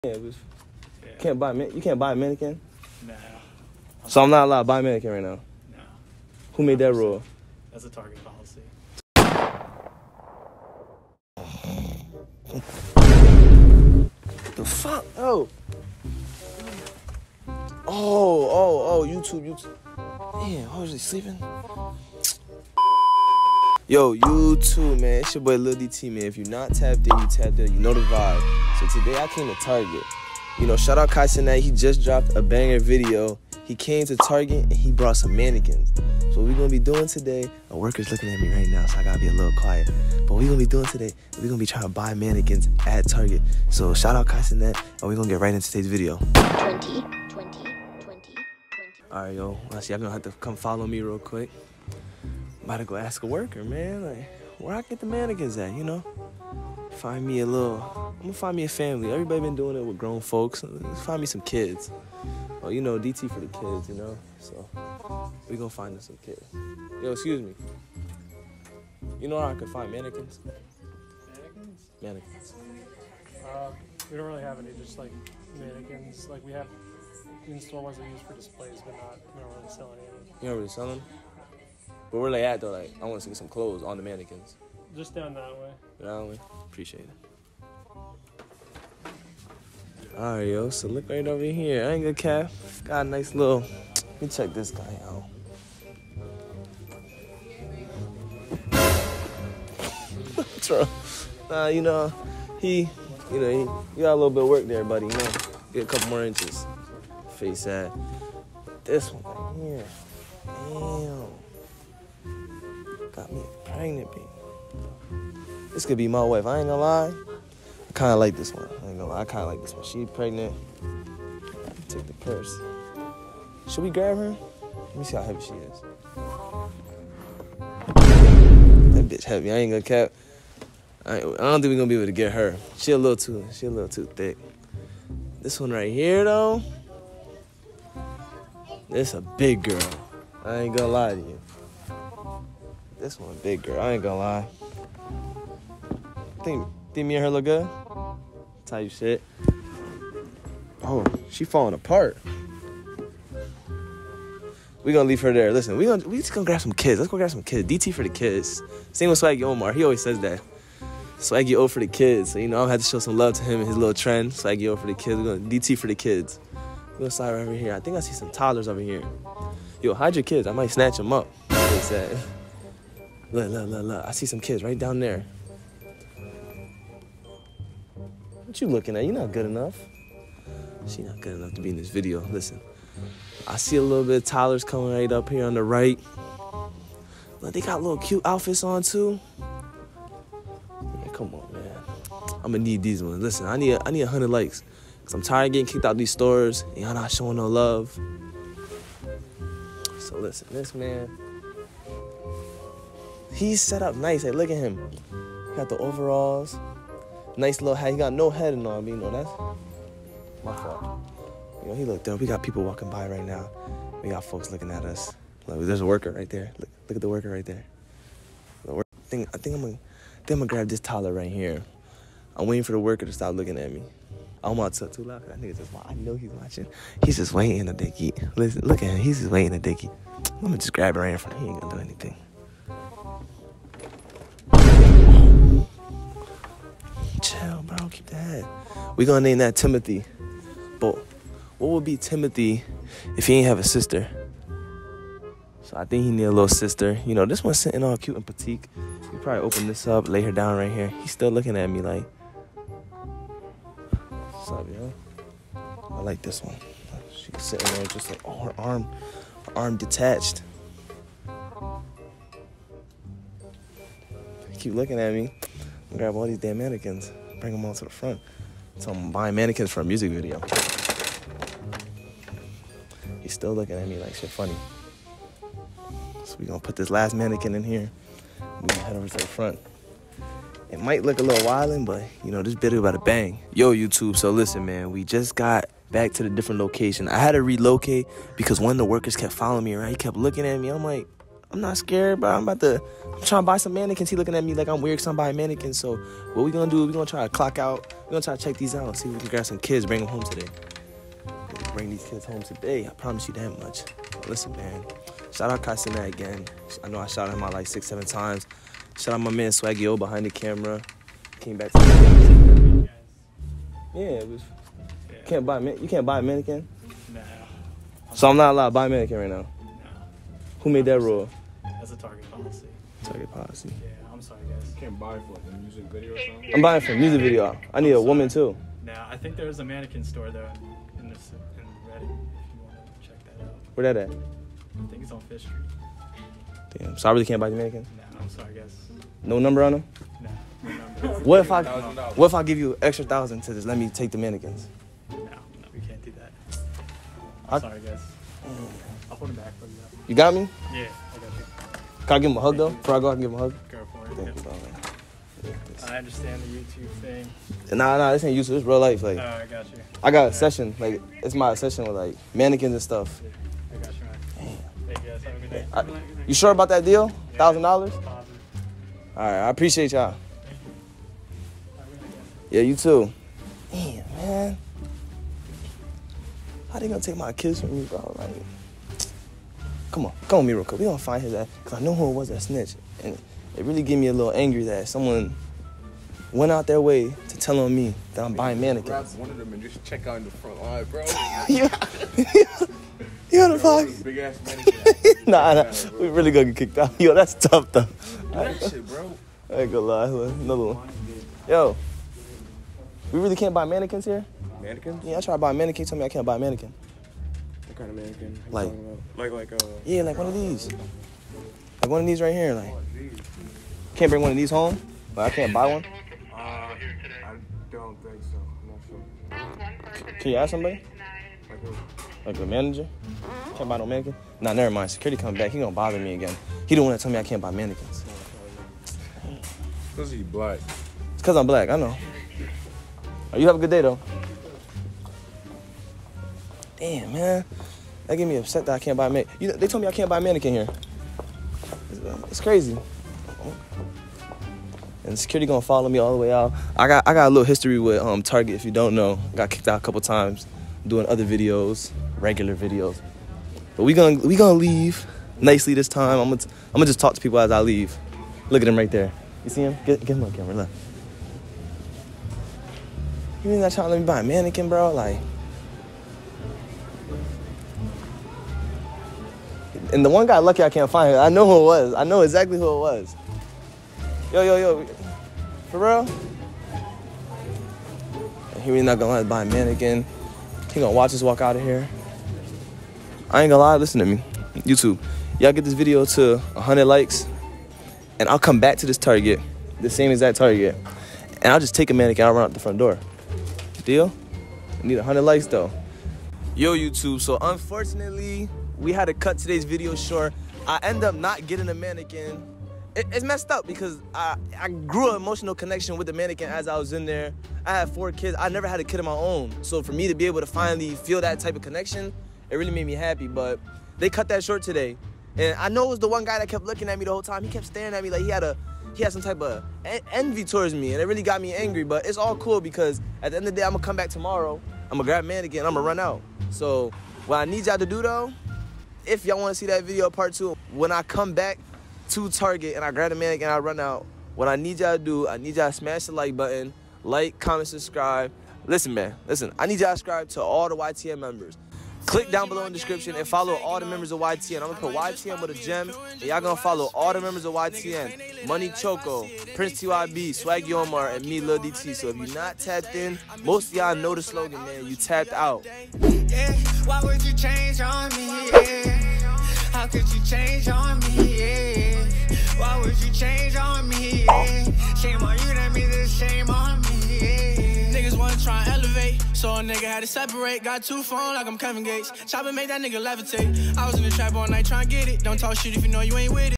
Buying Mannequins In Target! *THEY KICKED ME OUT*
TODAY I WENT TO TARGET TO BUY MANNEQUINS AND IT DIDN'T GO WELL SO I NEED YOU TO SMASH THE LIKE BOTTON AND ALSO FOLLOW THE YTN MEMBERS

SWAGGY ~

PRINCE ~

MONEY CHOCO ~